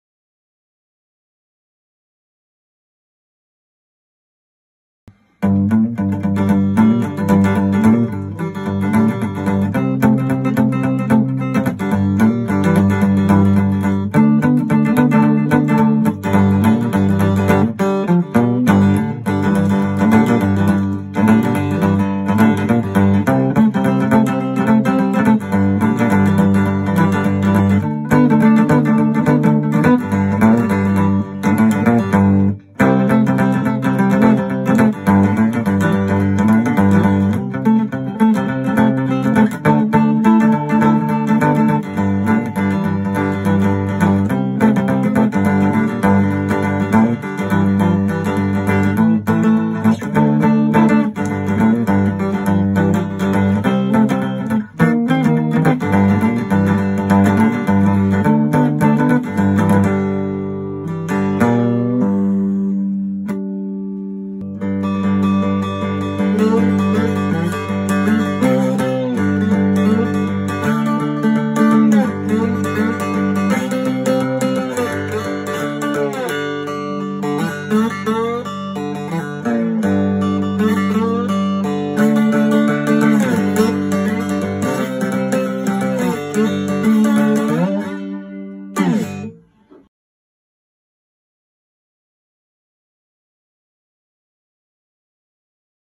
Build #27 "The Tabak Especial"
Mahogany neck, split coil humbucker with three way switch, 25" scale, Open G, Tabak Cigar Box